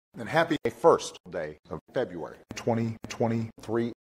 Happy First Day Of February 2023!
Barack Obama wishes you all a happy first day of February 2023!